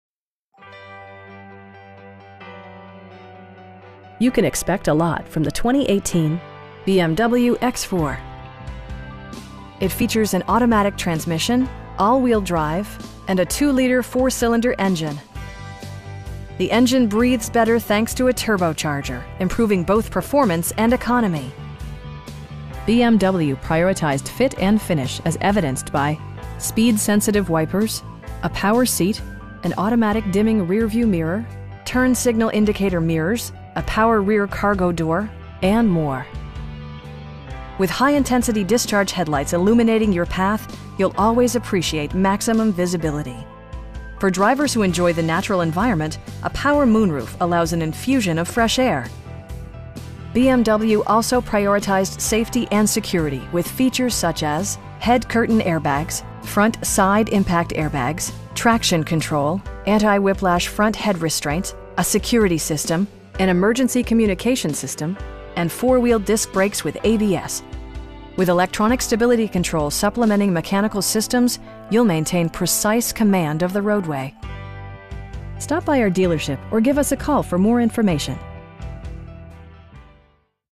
2018 BMW X4 xDrive28i in Seattle, WA 98134
BMW Seattle
1002 Airport Way S.

You can expect a lot from the 2018 BMW X4. It features an automatic transmission, all-wheel drive, and a 2 liter 4 cylinder engine. The engine breathes better thanks to a turbocharger, improving both performance and economy. BMW prioritized fit and finish as evidenced by: speed sensitive wipers, a power seat, an automatic dimming rear-view mirror, front bucket seats, turn signal indicator mirrors, a power rear cargo door, and more. With high intensity discharge headlights illuminating your path, you'll always appreciate maximum visibility. For drivers who enjoy the natural environment, a power moon roof allows an infusion of fresh air. BMW also prioritized safety and security with features such as: head curtain airbags, front side impact airbags, traction control, brake assist, anti-whiplash front head restraints, a security system, an emergency communication system, and 4 wheel disc brakes with ABS. With electronic stability control supplementing mechanical systems, you'll maintain precise command of the roadway. We pride ourselves on providing excellent customer service. Stop by our dealership or give us a call for more information.